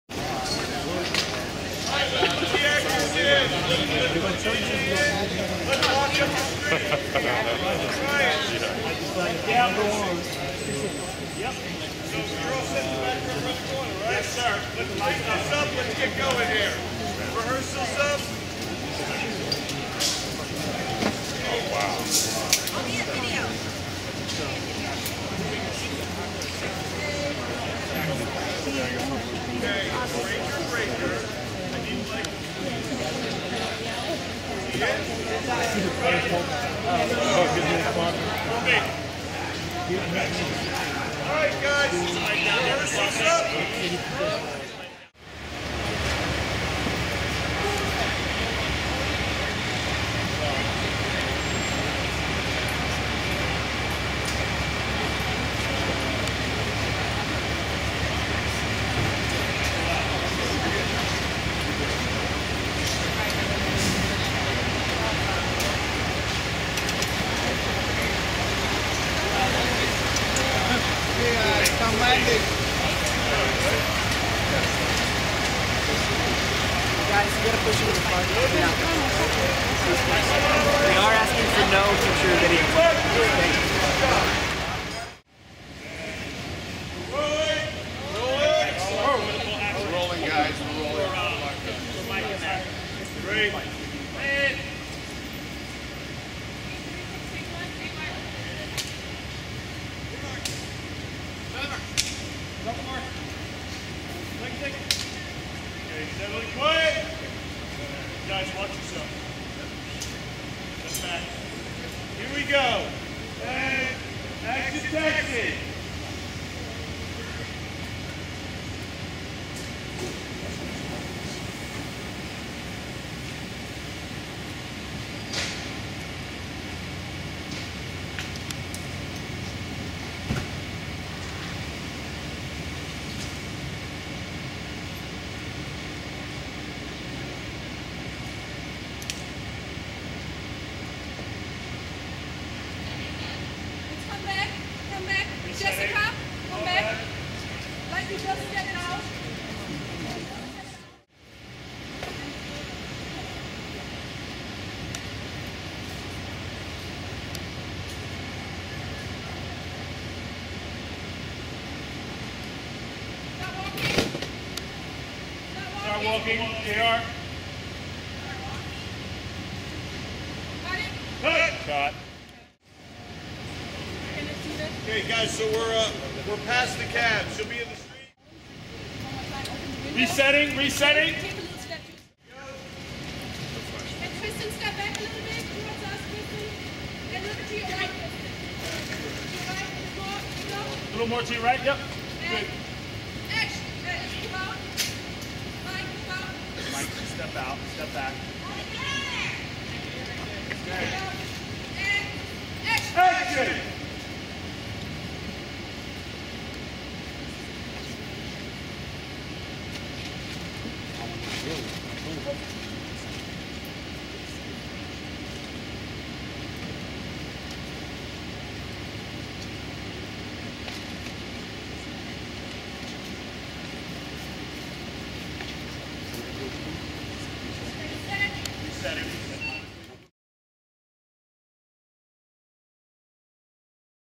Pure Fanzine Stuff
Ms. Krysten Ritter herself, in the making of A.K.A. Jessica Jones, outside Vazac's Bar on Avenue B and East 7th Street.